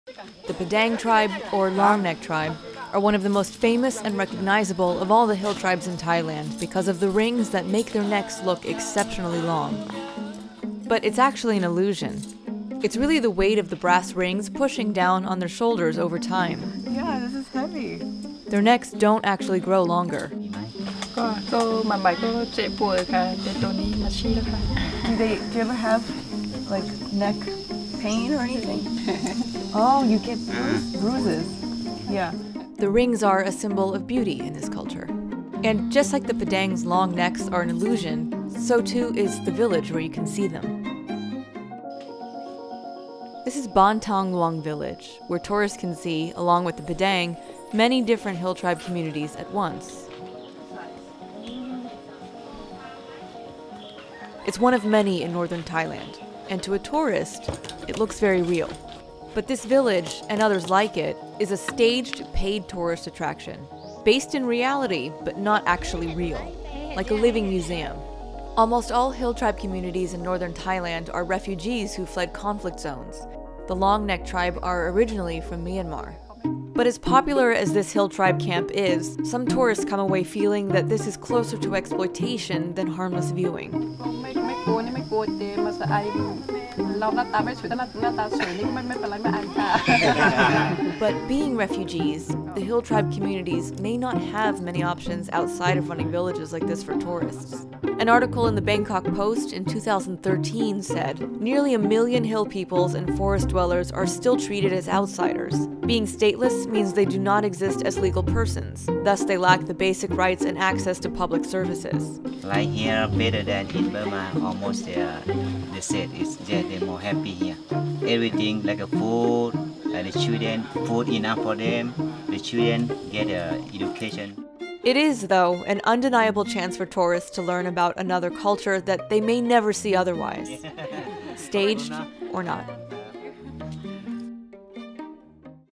Thailand's famous long neck tribe people don't actually have long necks at all
Thailand's famous long neck tribe are called the Padaung, but despite their name, they don't actually have long necks. The rings create an illusion. Circa's travel journalist, Nathalie Basha (@TheTravelMuse) spends a day with the well-known hill tribe in Chiang Mai, Thailand.